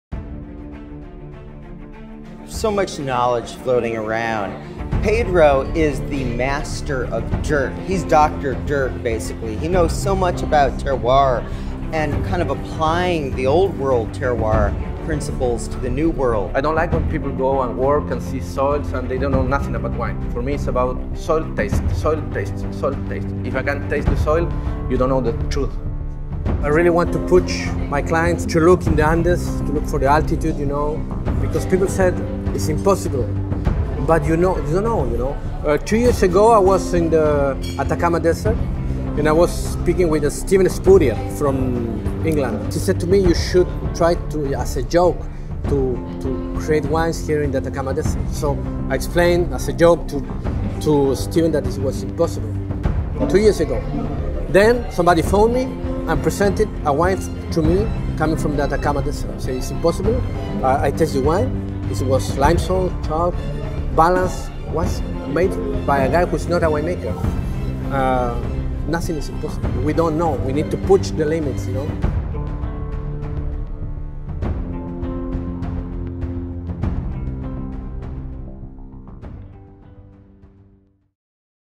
Wines of Chile "Vinos de Terroir" Tasting - Part 3
Mark Oldman, Wine Author and Pedro Parra, PhD and Terroir Expert, focus on pushing the limits of terroir at the Vinos de Terroir tasting in New York City.